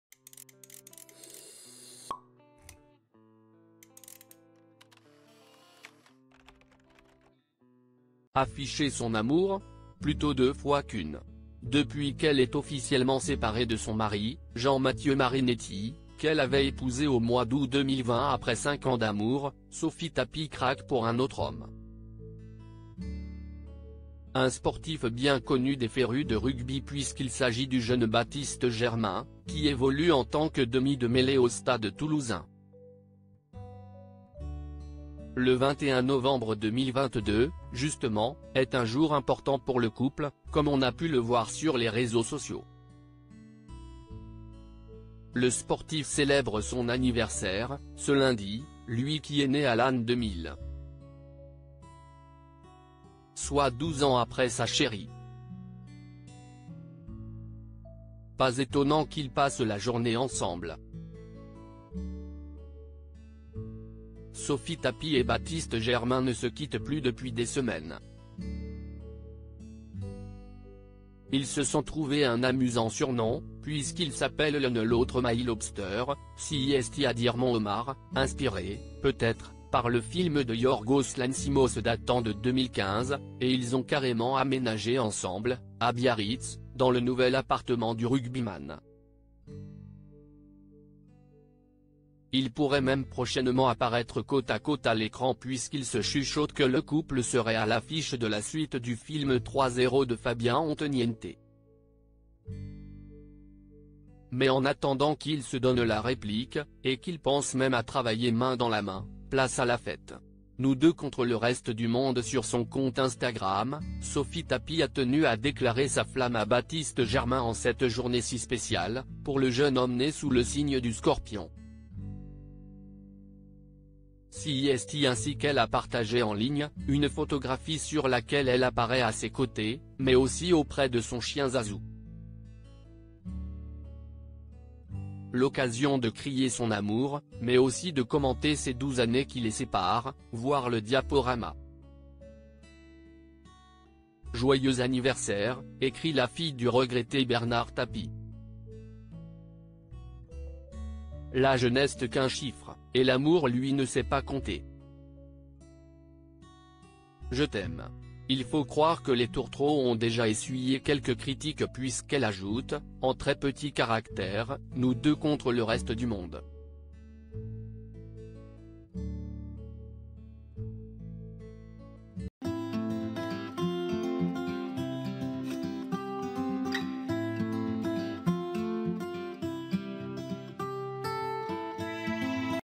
Sophie Tapie en couple avec un sportif bien plus jeune qu'elle, elle évoque enfin leur différence d'
Sophie Tapie en couple avec un sportif bien plus jeune qu'elle, elle évoque enfin leur différence d'âge !